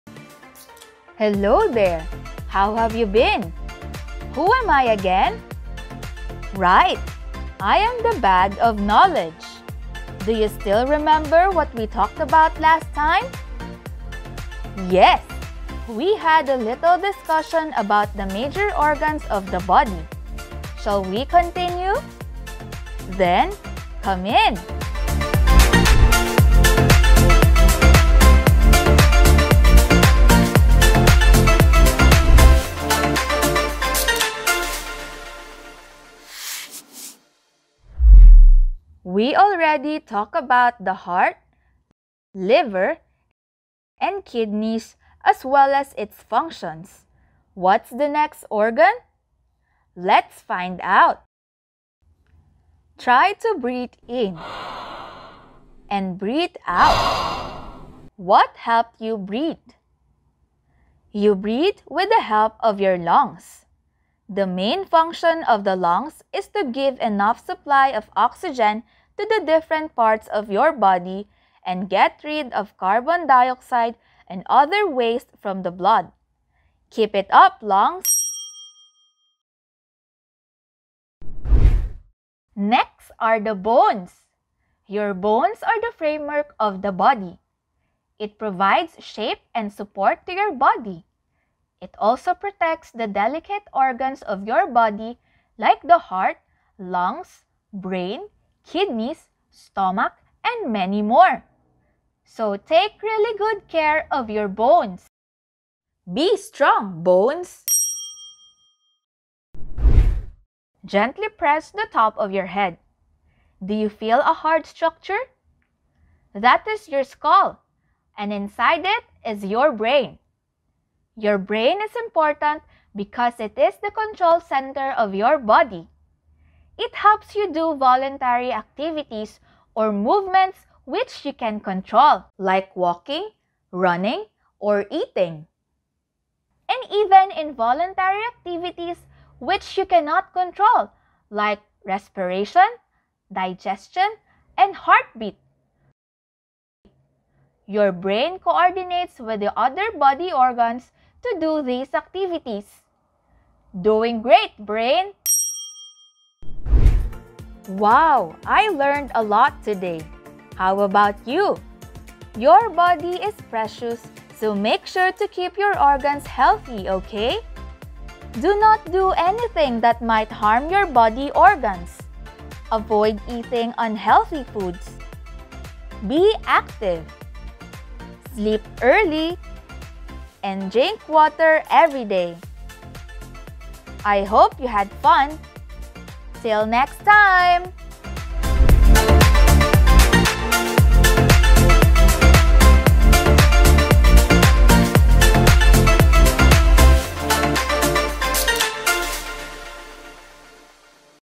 [3-MINUTE LESSON] Science: Teamwork Makes Your Body Work (Part 2)
The second part of our discussion on "Teamwork Makes Your Body Work" is here!